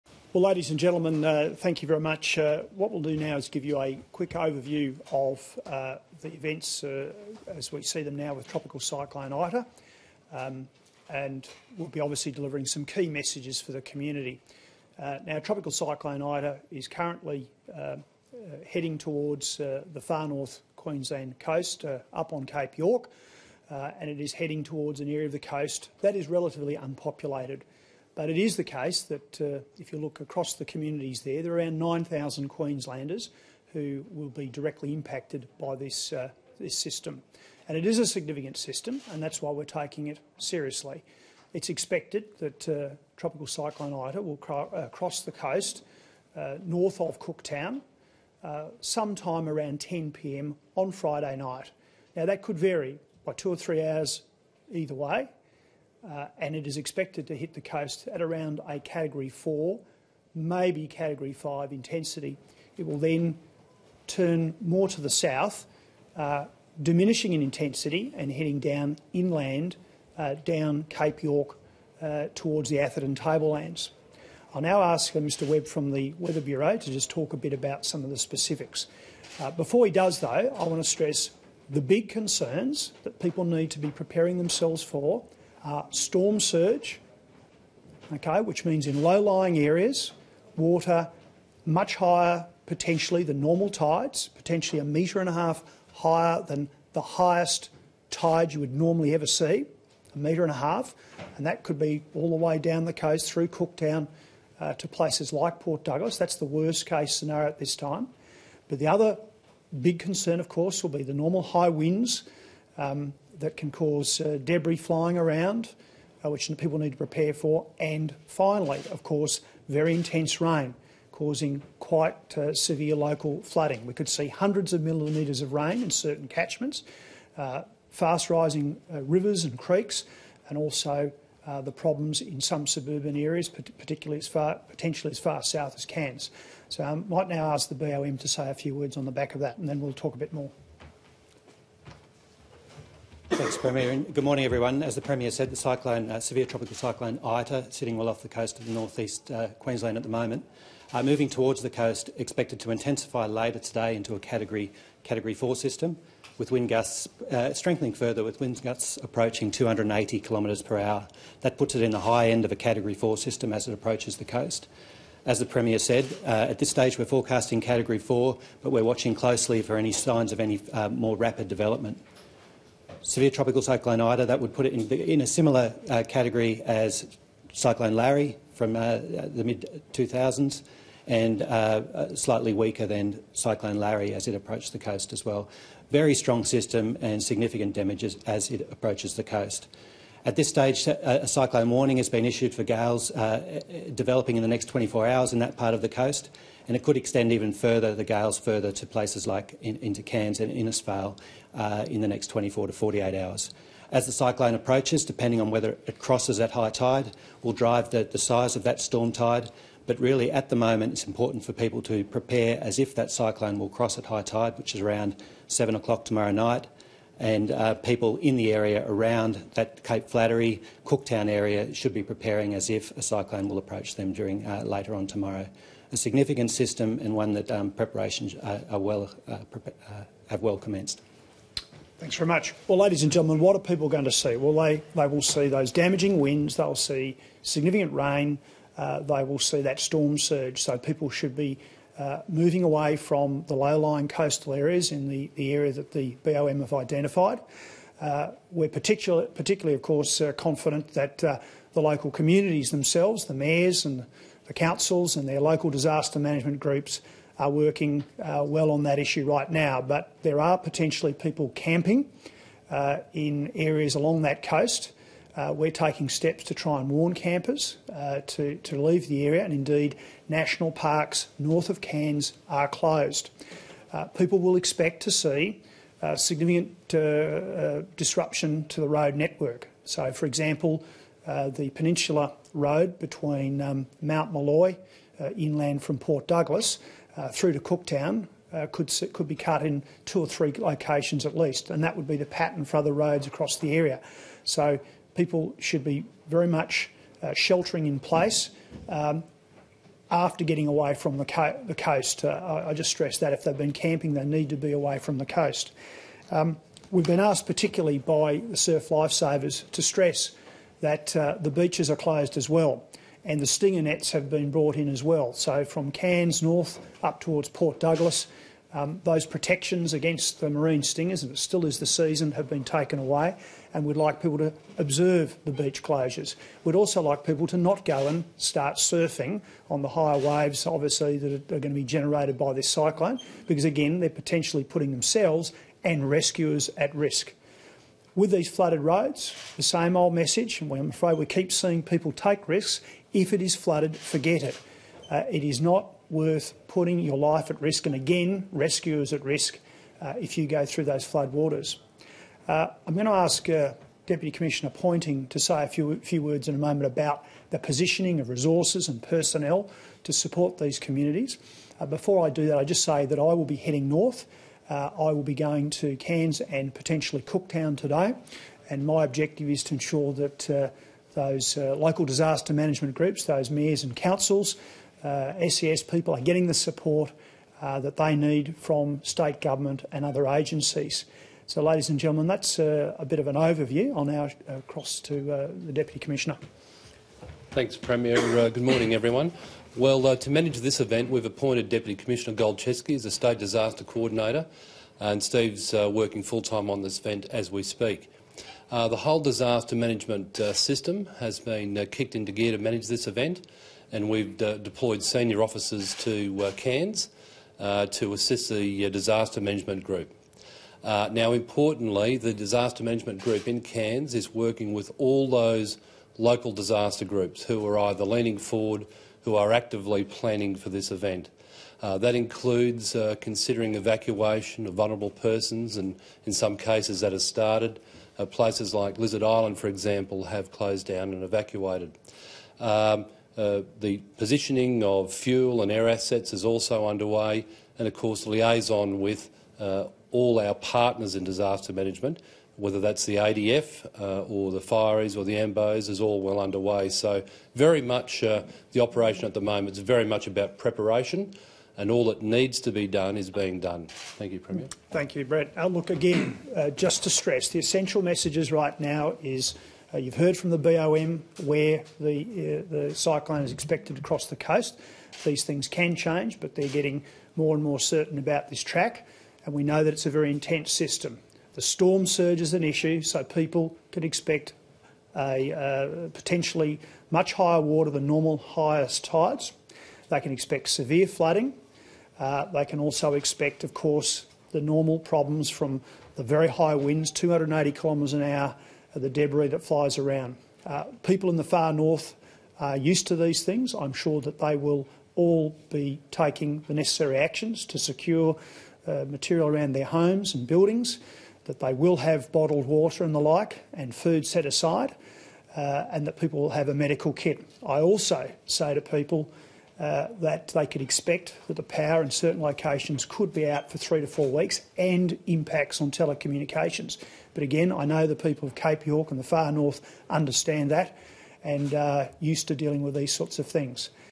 Campbell Newman & Qld emergency services urge preparation for Cyclone Ita
10/04/14 Far North Queensland is being urged to prepare for damaging winds and high seas as Cyclone Ita approaches the coast and is forecast to cross as category four system.